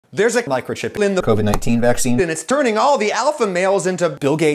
Adam Ragusea thinks Bill Gates is behind the pandemic
Please vaccinate your kids!
This is just a meme video, no disrespect towards Adam